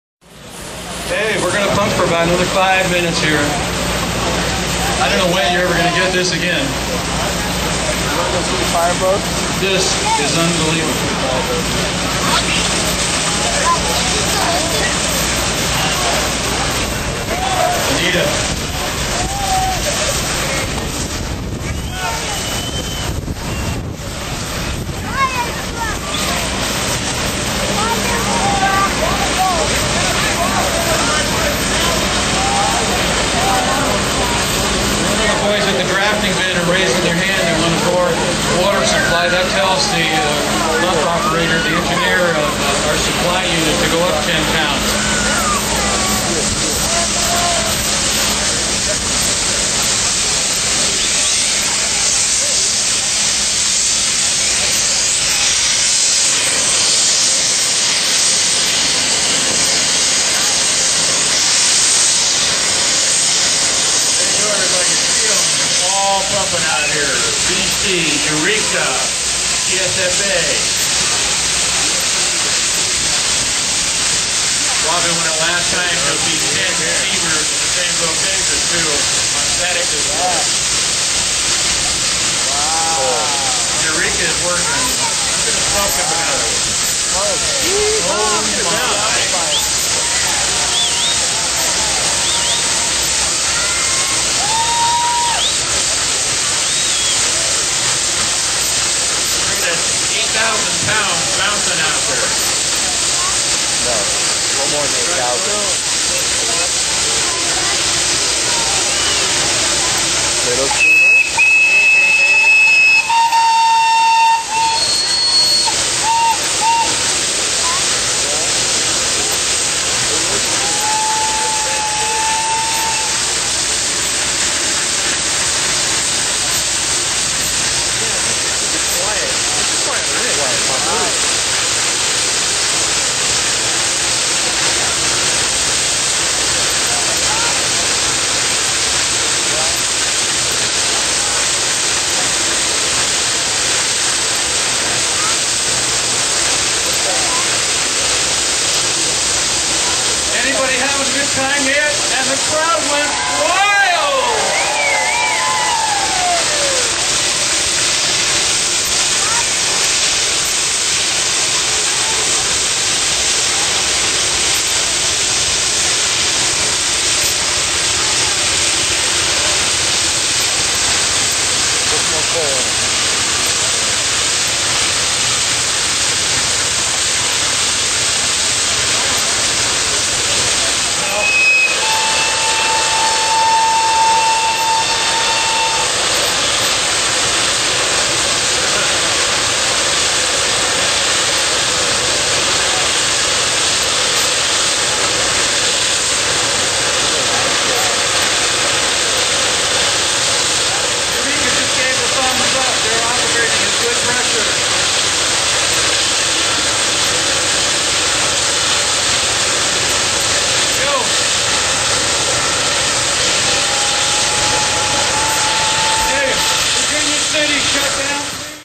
Eureka Steam Fire Engine
Eureka's 1904 metropolitan Extra First Class Steam Engine during a pumping demonstration at San Francisco's 1906 Great Earthquake and Fire Expo in April 2006.